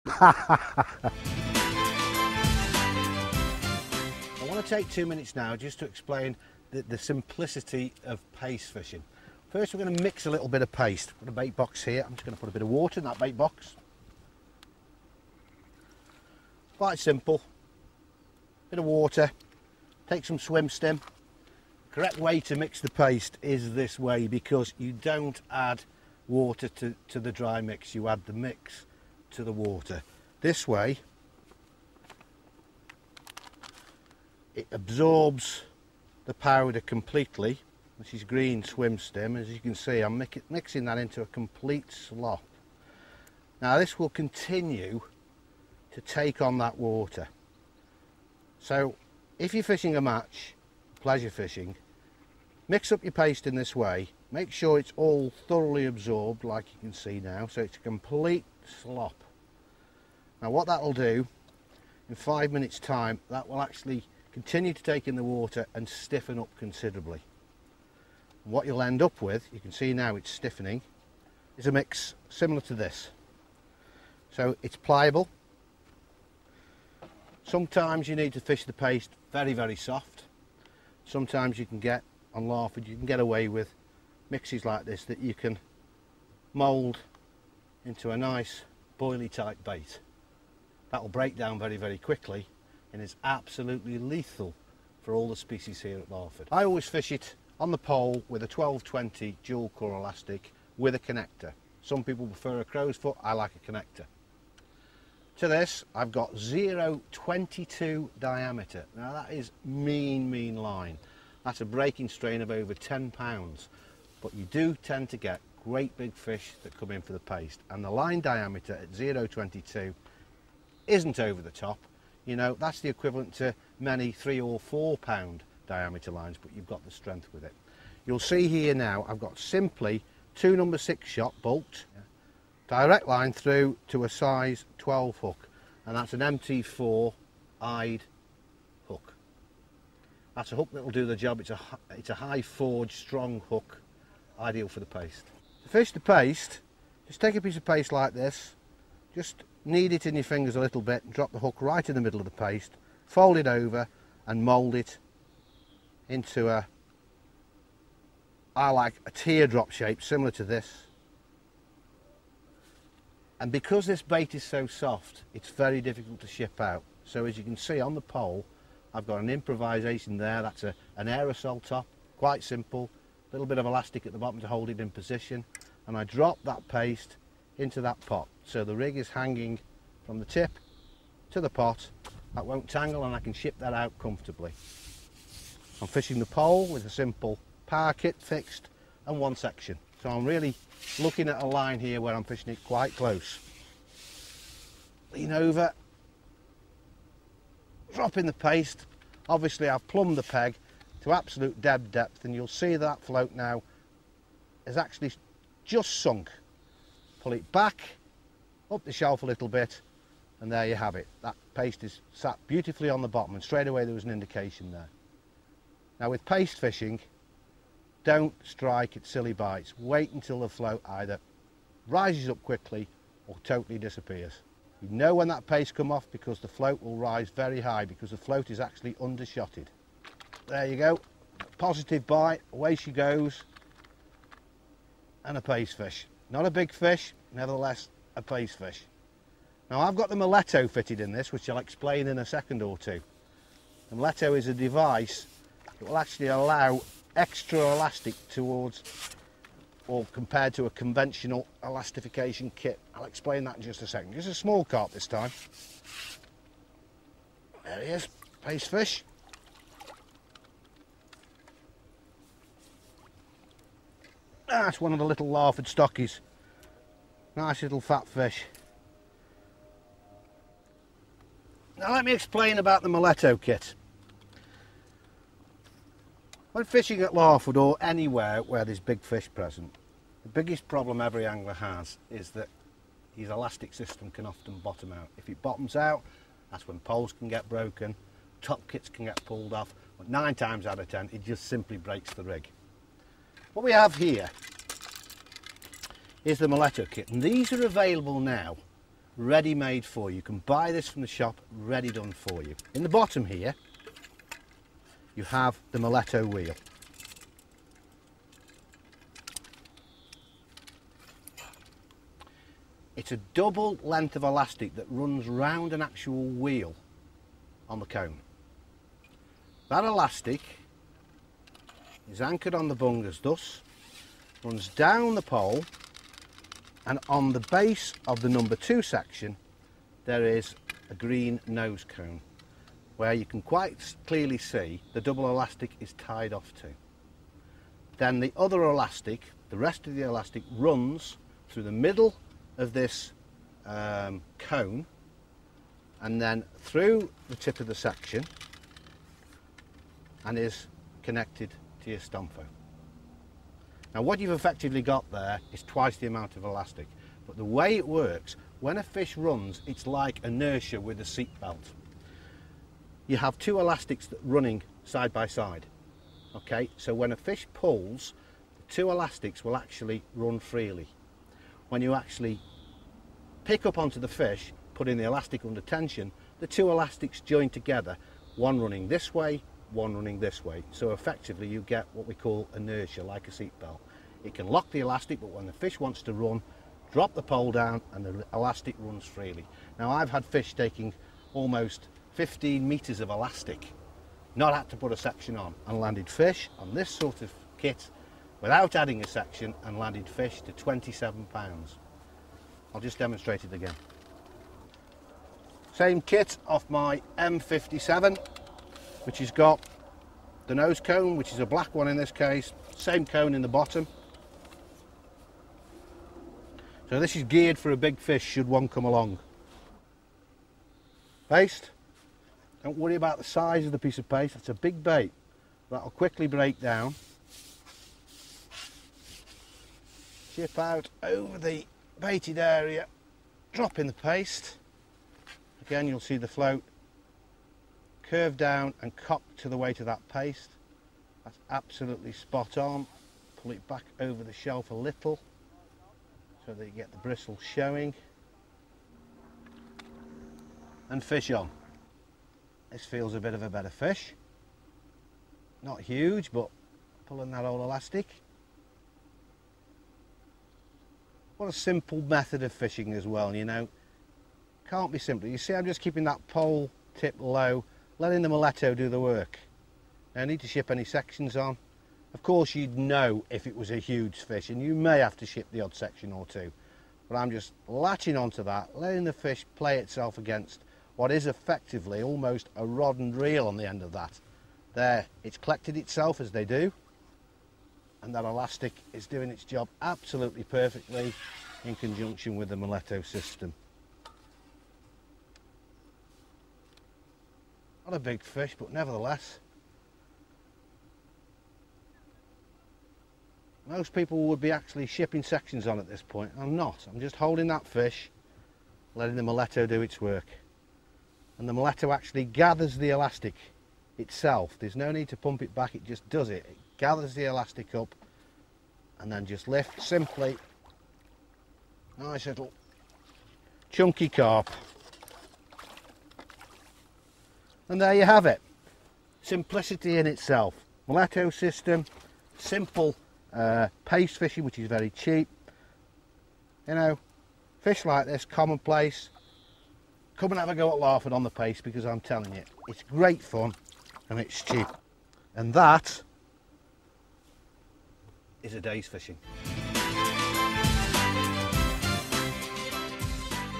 Paste Fishing & Muletto demonstration
Maver UK MD Phil Briscoe demonstrates paste fishing as well as the Muletto power kit.